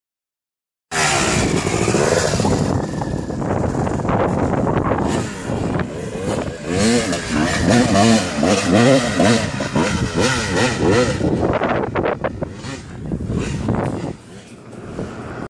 Kx 250 2 stroke steep hill climb
2004 kx250 hill climb